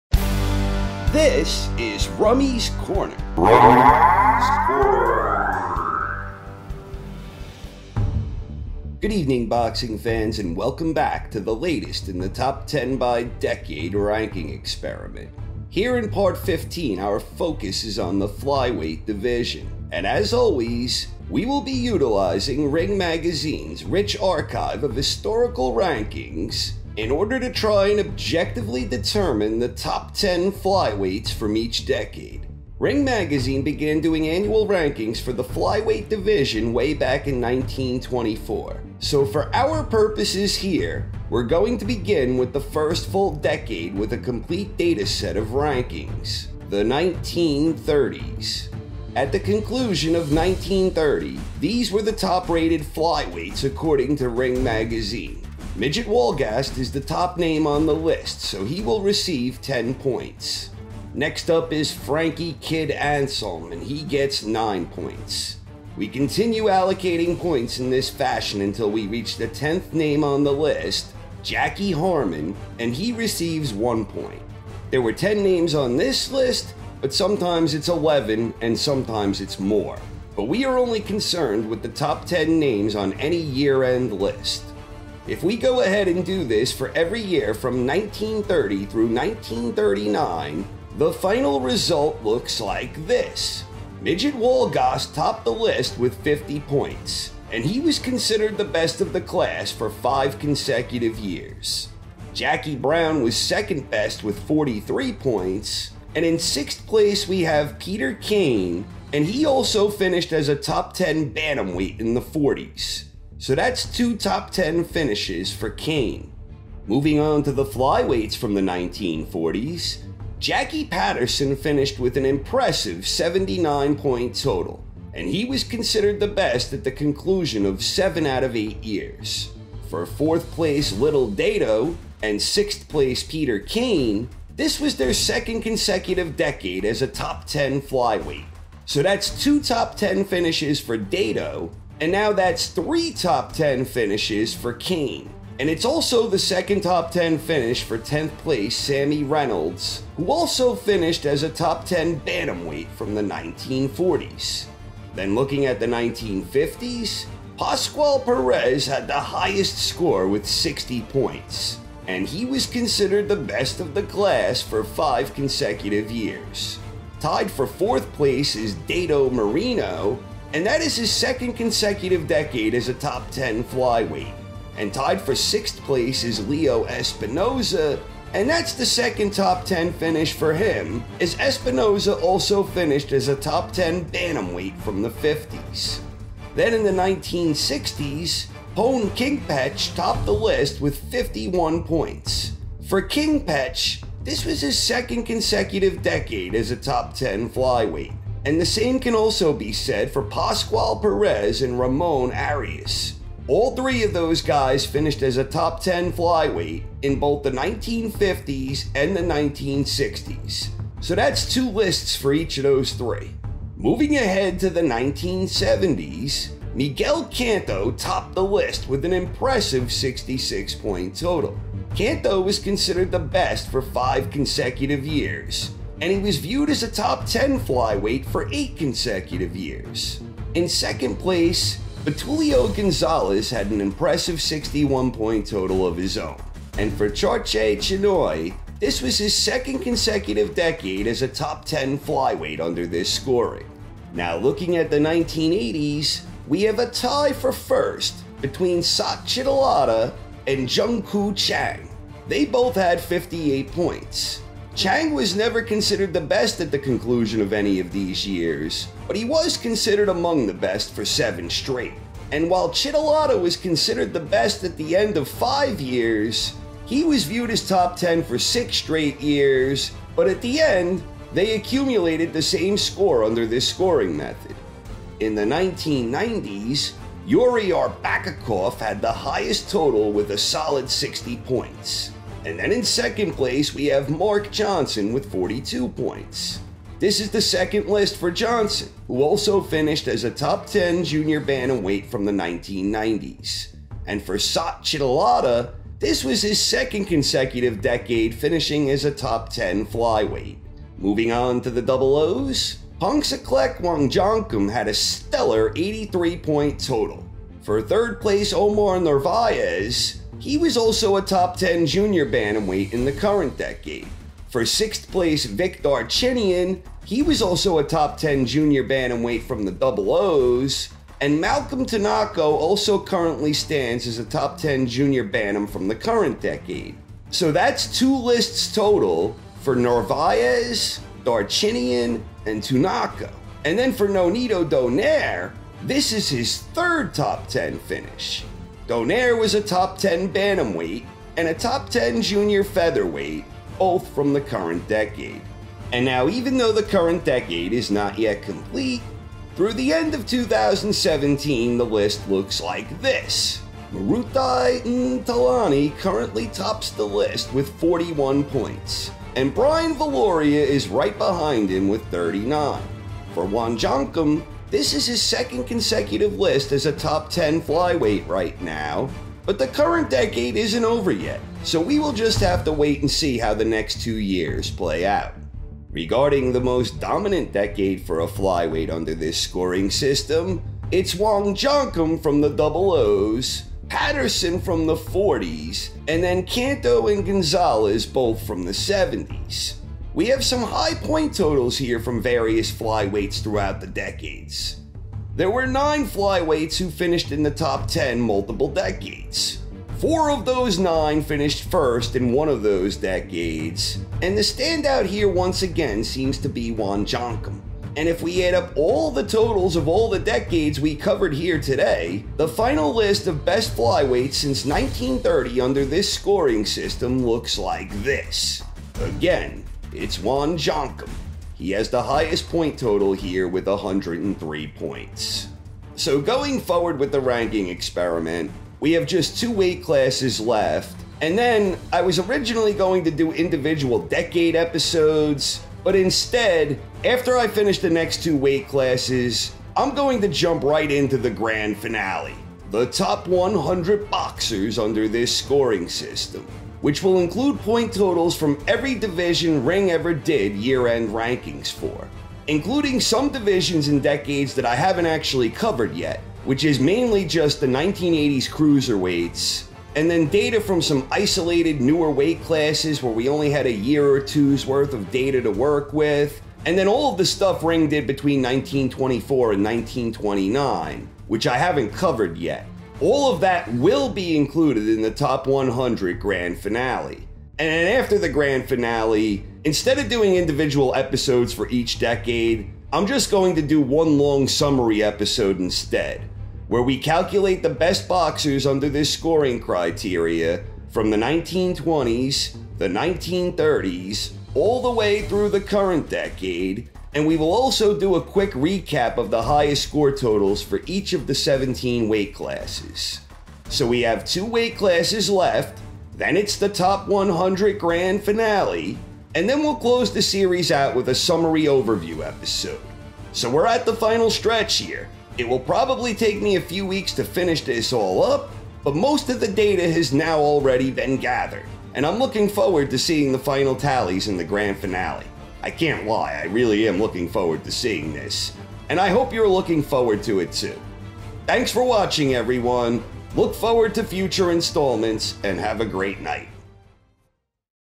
Top 10 Flyweights by Decade
Top 10 by Decade: Part 15 Flyweights

A ranking experiment which attempts to objectively rank the best flyweight  boxers across different decades.